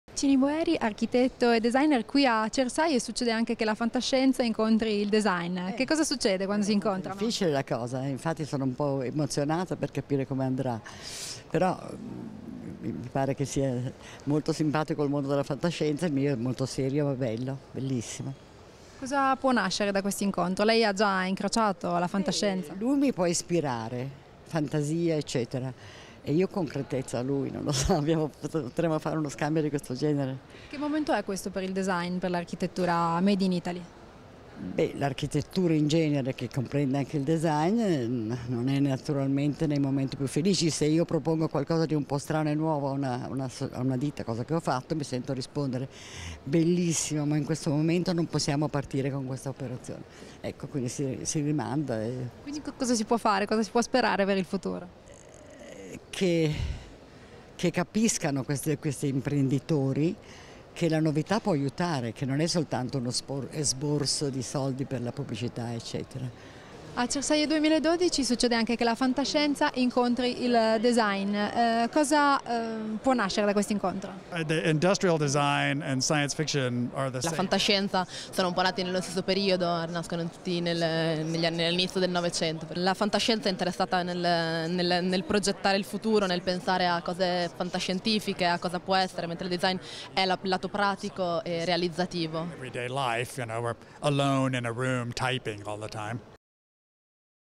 Cersaie 2012: incontro con Cini Boeri e Bruce Sterling - video by Telesanterno
In occasione di CERSAIE 2012 incontro tra la designer italiana Cini Boeri e lo scrittore di fantascienza Bruce Sterling: quando il design incontra la fantascienza - video by Telesanterno